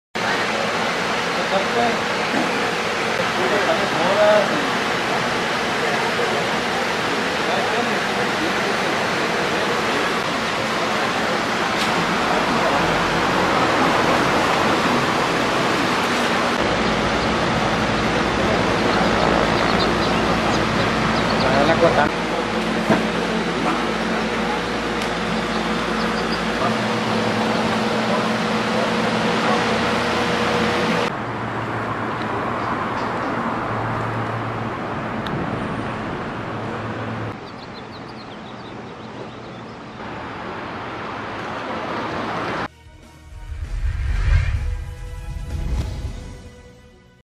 Sindicalizados cumplen una semana en huelga de hambre
Guasave, Sin. Son siete los manifestantes que permanecen instalados en improvisados tendidos, bajo una carpa afuera de las oficinas de la Jumapag. Aunque el semblante es de cansancio, sostienen que no se pueden retirar pues hay acuerdos que tomaron en grupo y se tienen que respetar.